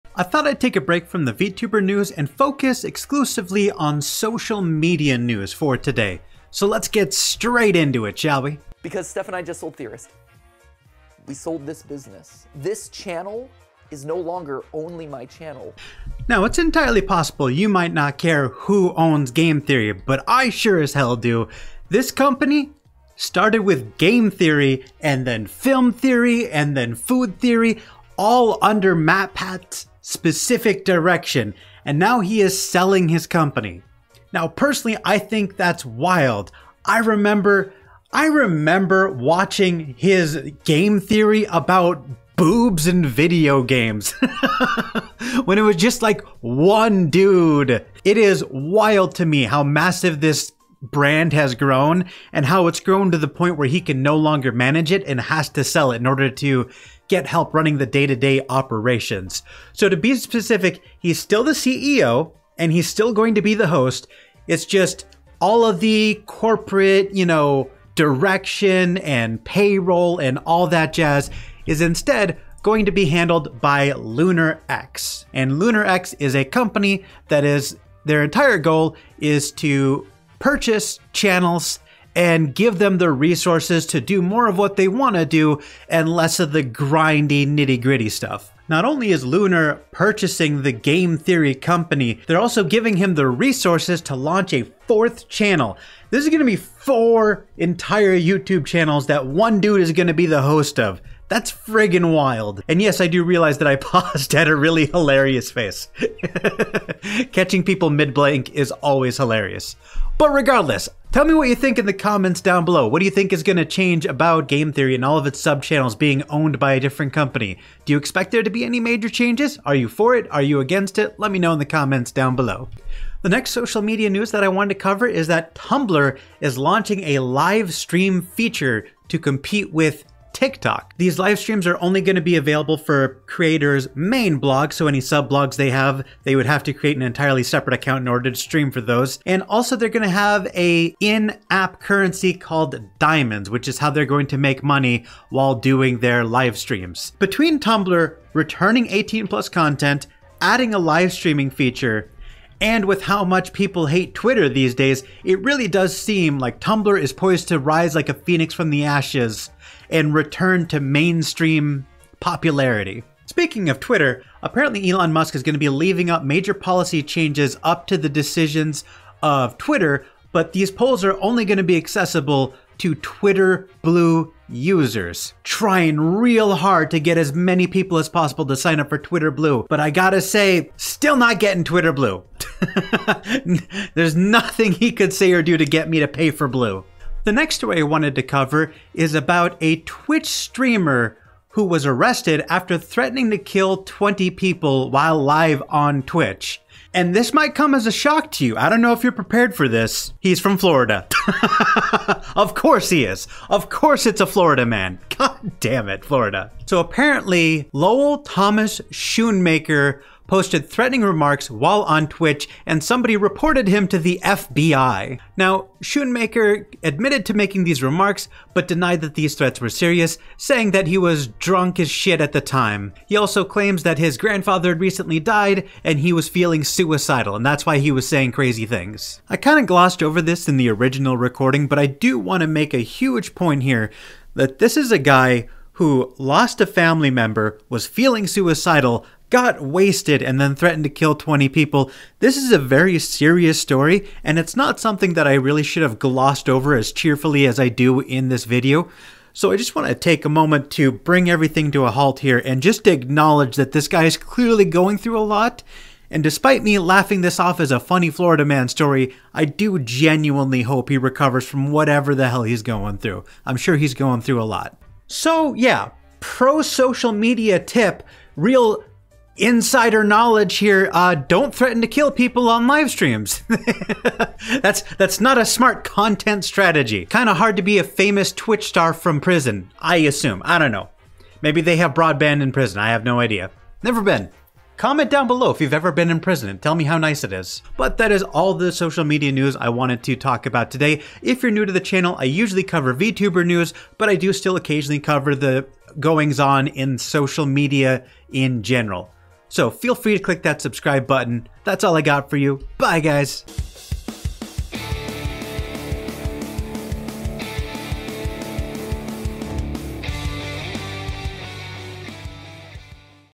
MatPat sells Game Theory, Tumblr adds livestreams, Florida Man on Twitch
0:00 intro
0:08 MatPat sells Game Theory
2:02 Tumblr adds livestreaming
2:43 Elon Musk making company decisions through Twitter polls
3:06 Florida Man on Twitch